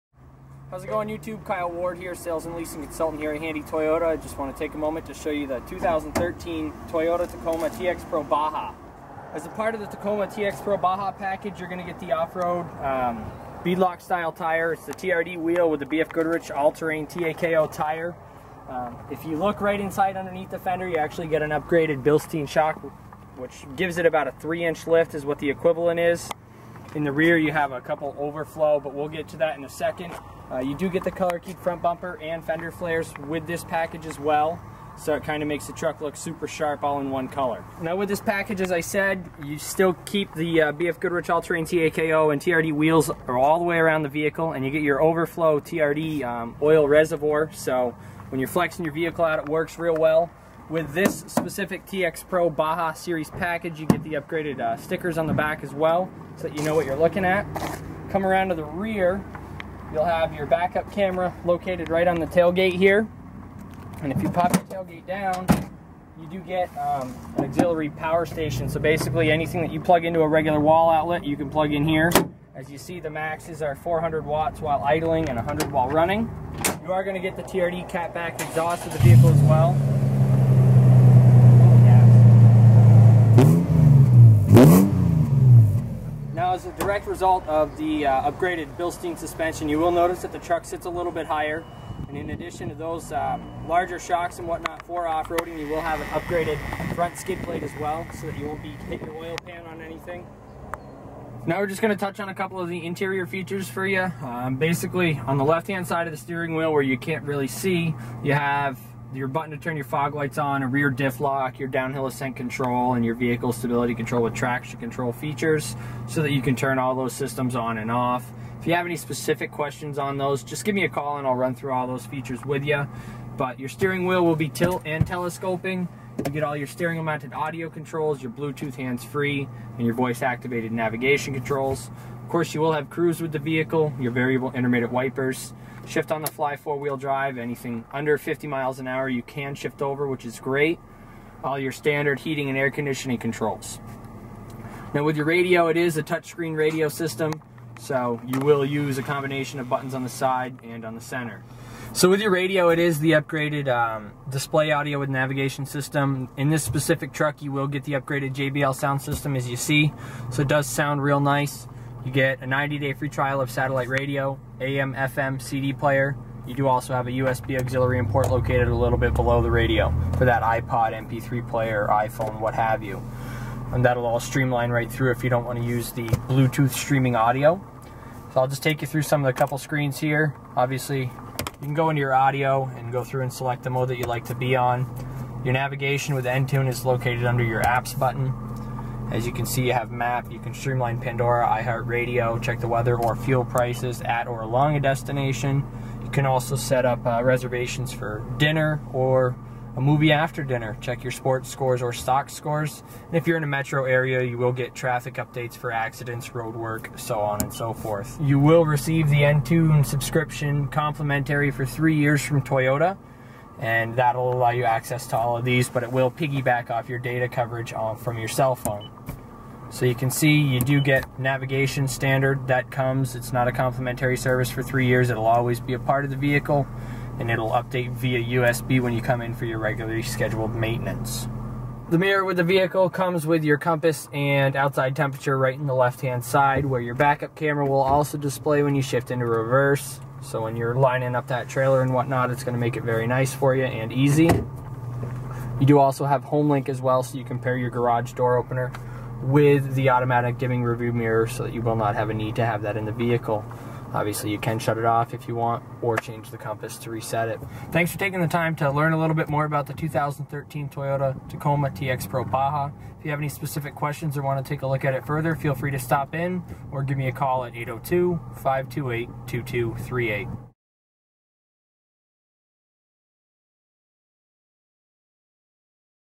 2013 Toyota Tacoma TRD TX Pro Baja Series
Handy Toyota Salesman Kyle Ward gives a walk around of the impressive Baja Series Tacoma.